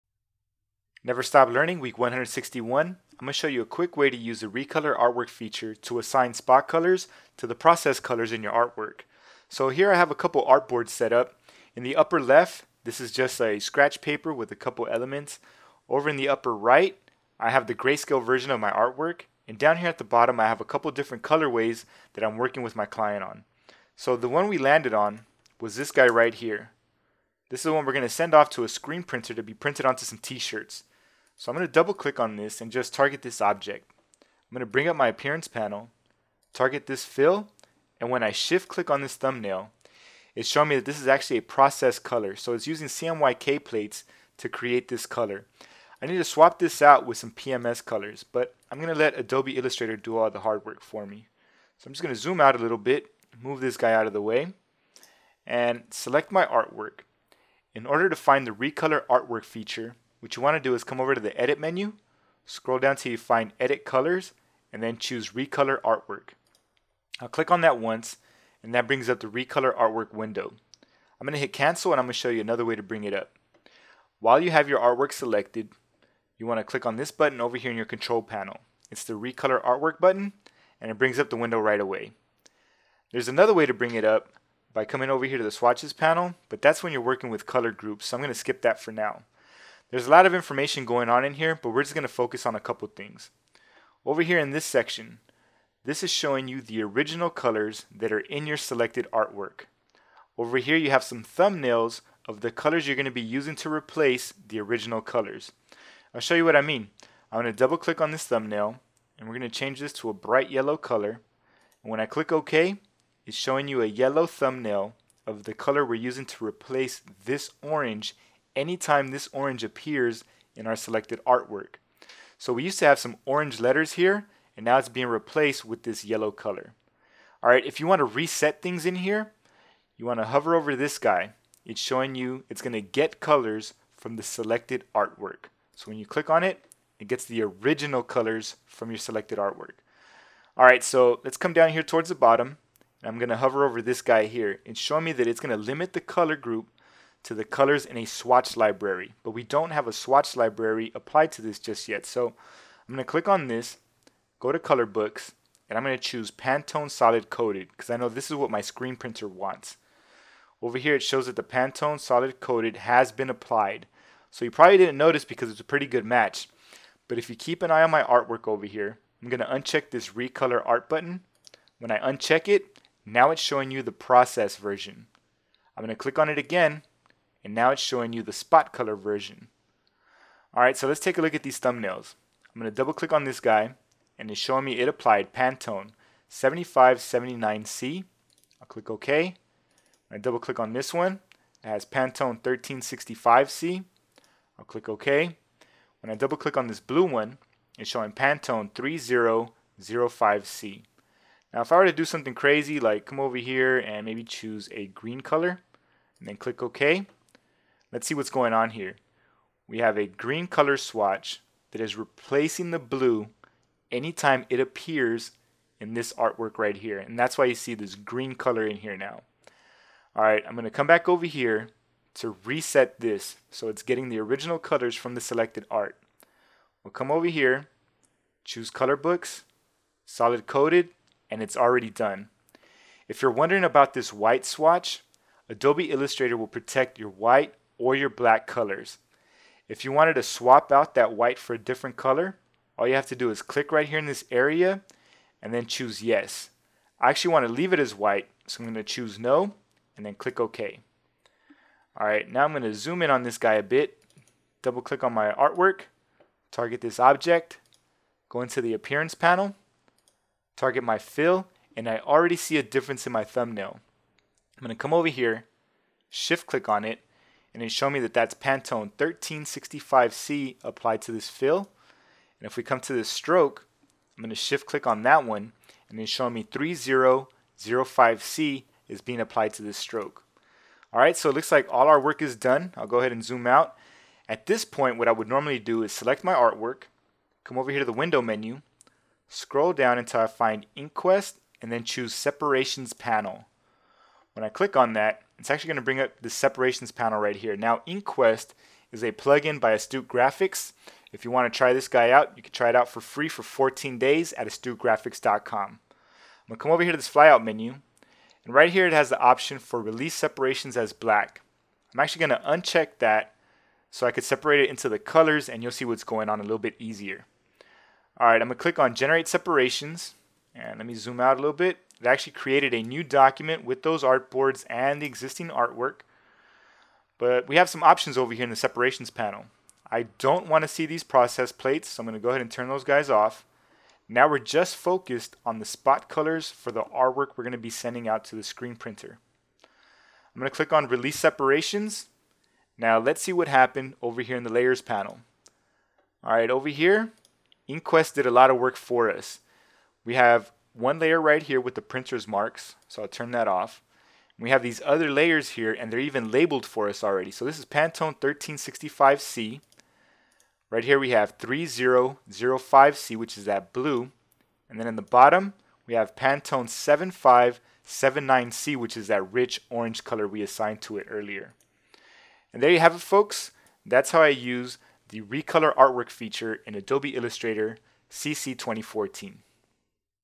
Adobe Illustrator CC 2014: "Recolor Artwork: Pantone Spot Colors" N.S.L: WEEK 161
I'm not too familiar with this function, but the more I learn about it, the more I like it. In this video, I'll show you a quick way to assign matching Pantone colors to your process colored artwork. When I send out artwork to be printed, they always ask that I have the Spot colors in the artwork already. If you work with process colors and need a quick way to change them out with the right PMS color, this is the way to do it. You can do this via the Recolor Artwork in Adobe Illustrator. This is not a new feature but it is new to me =D

At the end of the video you'll see me use a plugin called InkQuest by Astute Graphics. This plugin allows you to automatically separate your colors into individual layers. You can try it out here for free for 14 days: